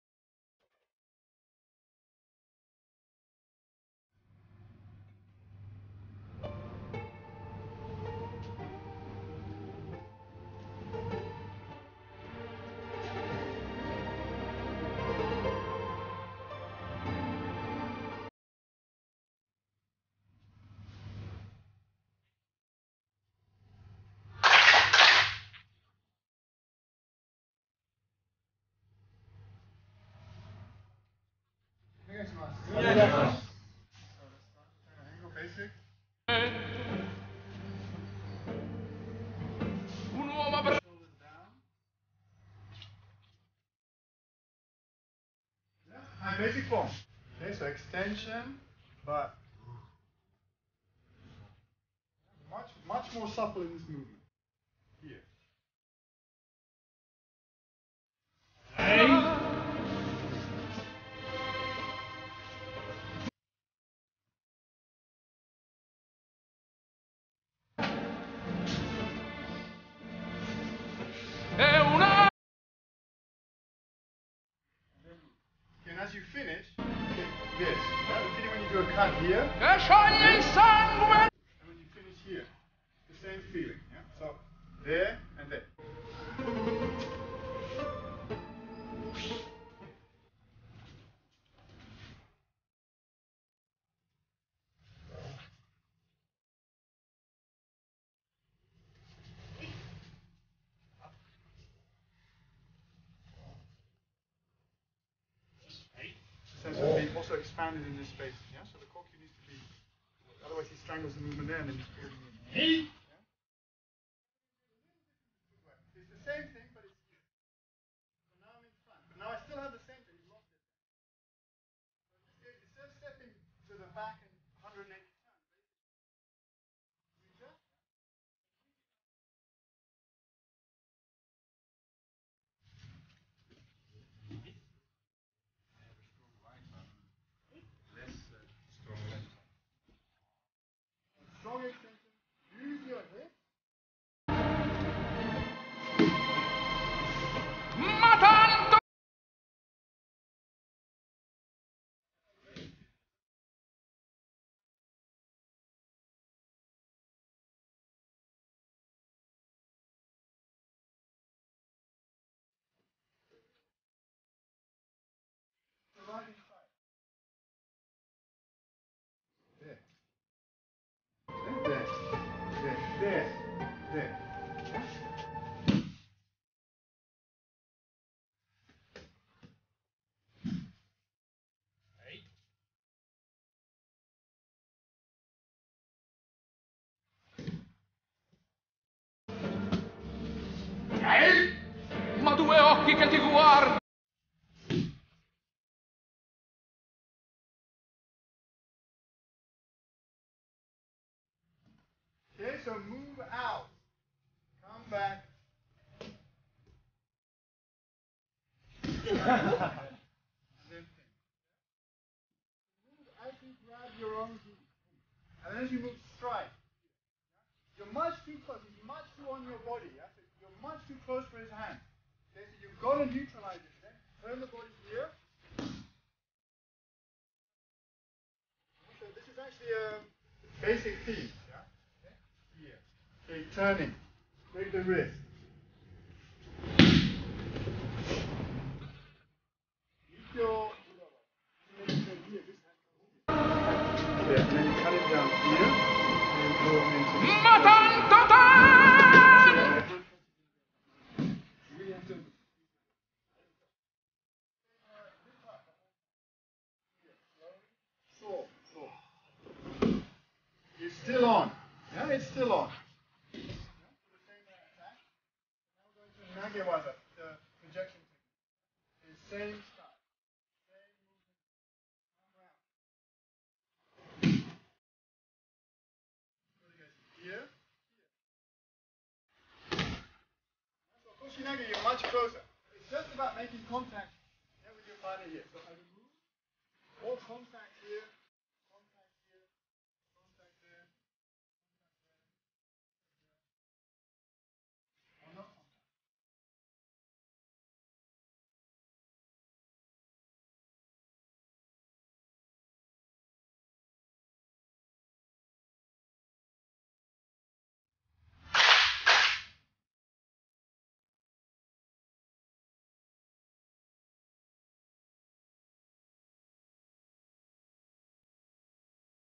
Traditional Aikido - Seminar with Lewis Bernaldo de Quiros
A two day seminar with Lewis Bernaldo de Quiros, 6 Dan, in Aikido Alive London's dojo in June 2009. Kihon Waza; basic techniques.